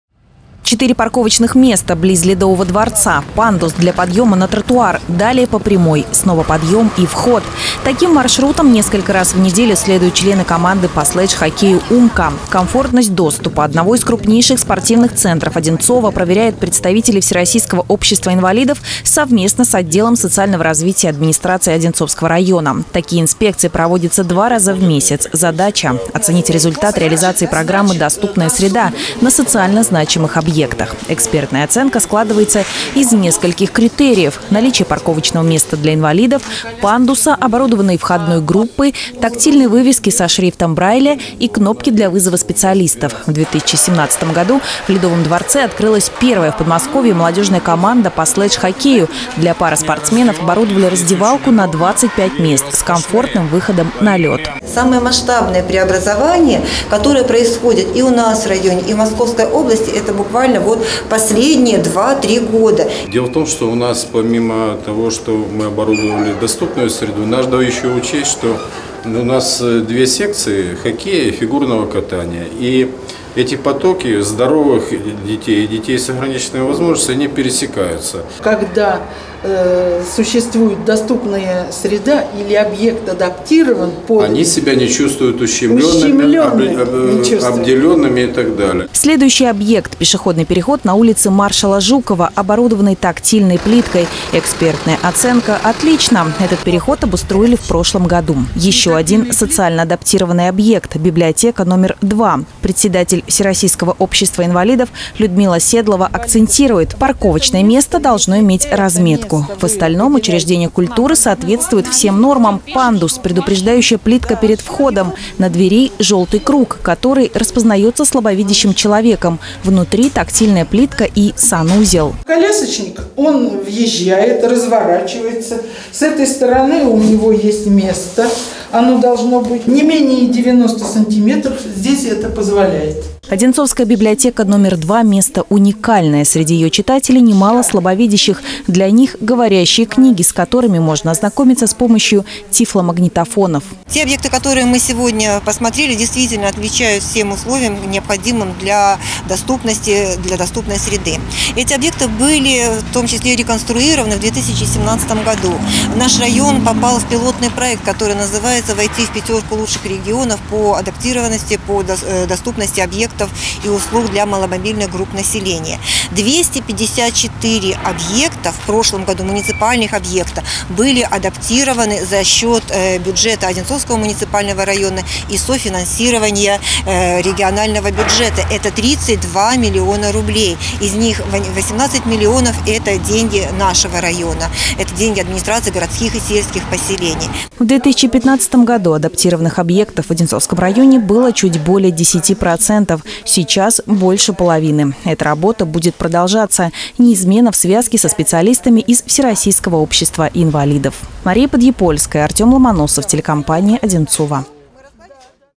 Инспекция социальных объектов прошла в Одинцово
Насколько успешно проходит реализация программы «Доступная среда» в Одинцово? В центре внимания у инспекторов были ледовый дворец «Армада», пешеходный переход на улице Маршала Жукова и Одинцовская библиотека №2.